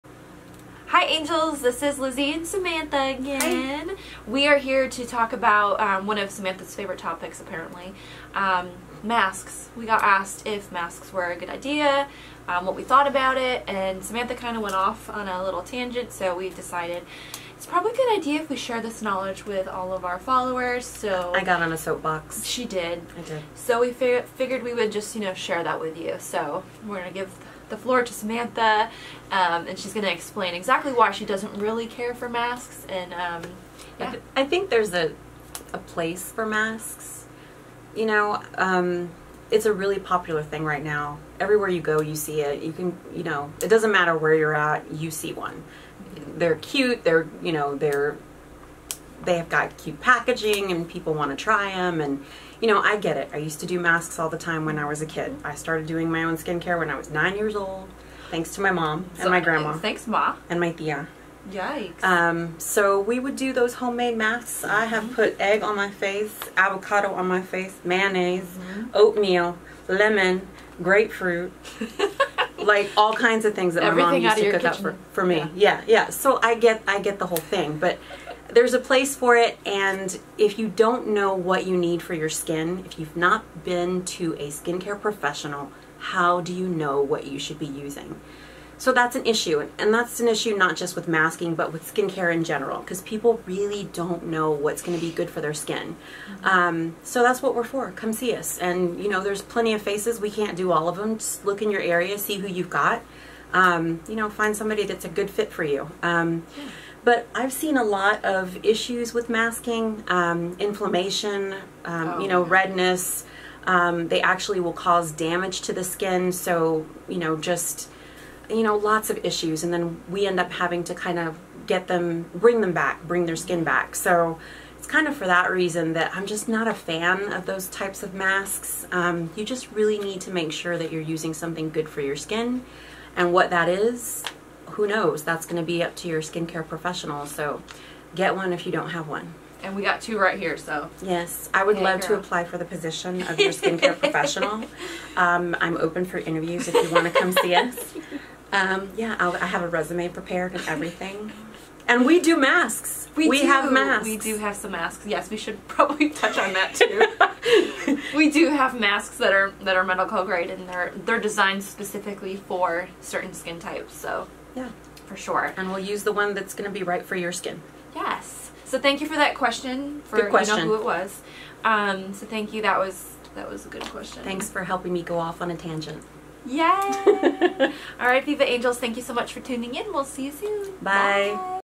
To mask or not to mask?!
This Skin Tip Tuesday we are talking about our thoughts on the ever popular at home MASKING!

While we love the idea of sitting around with girlfriends, drinking a glass of wine, they are not made for everyone.